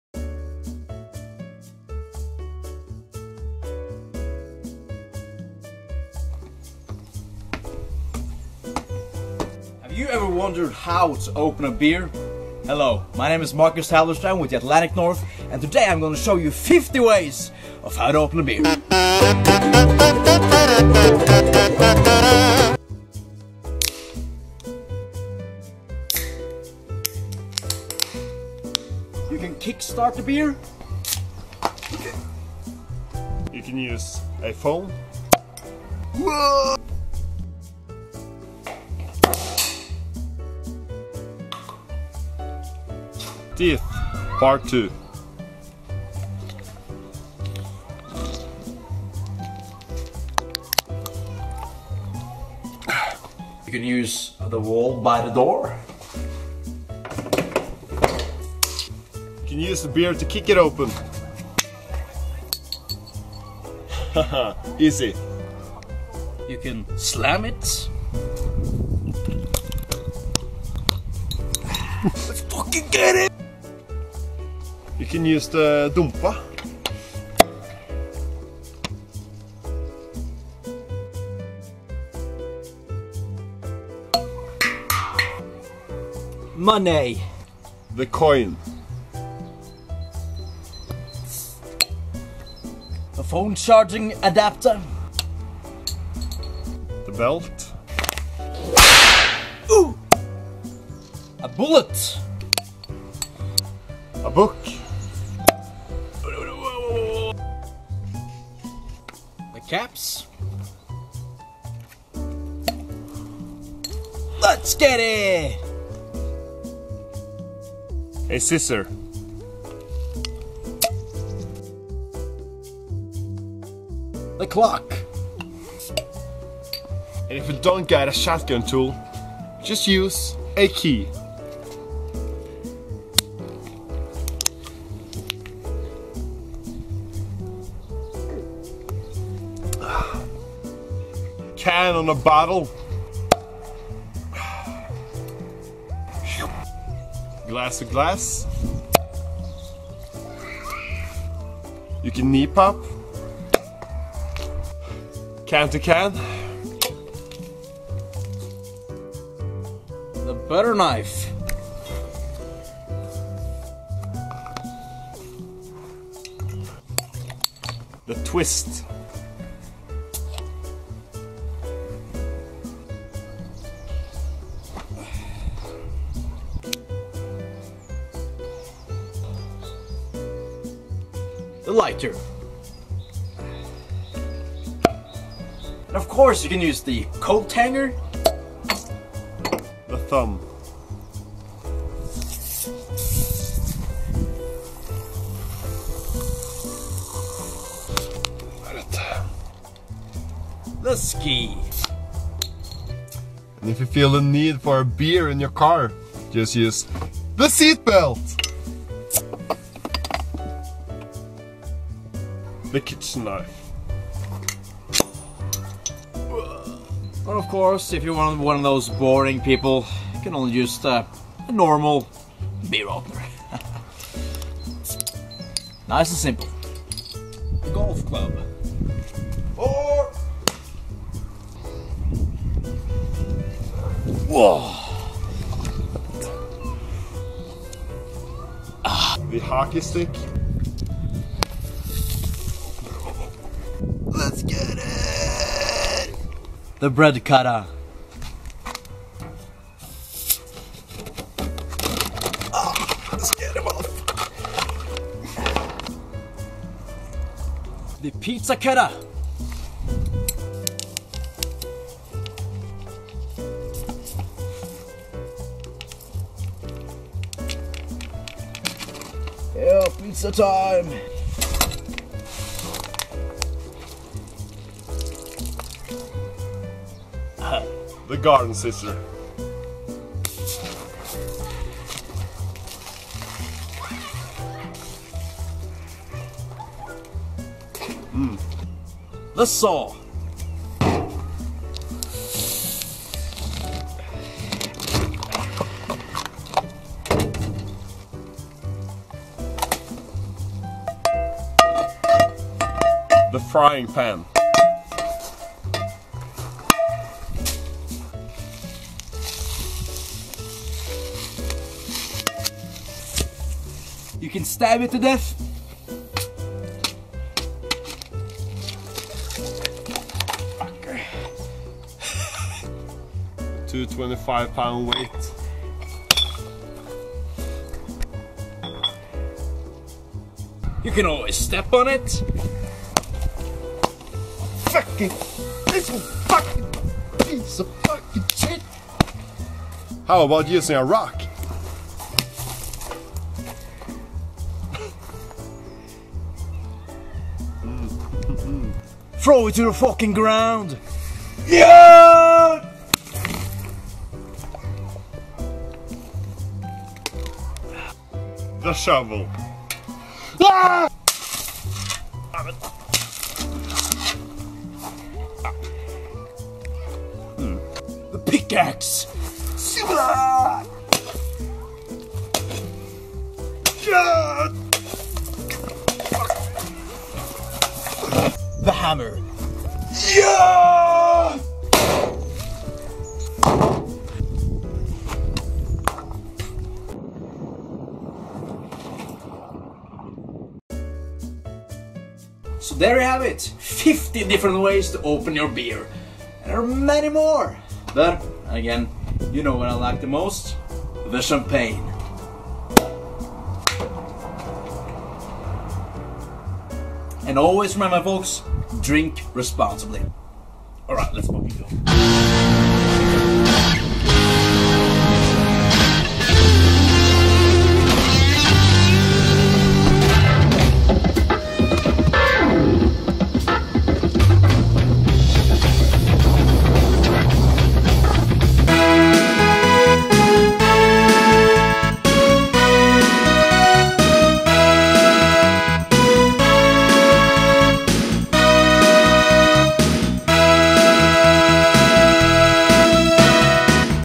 50 Ways to Open a Beer!
Marcus Halberstram & co shows you 50(!) ways to open that beer! wow, that is like, you know, yeah.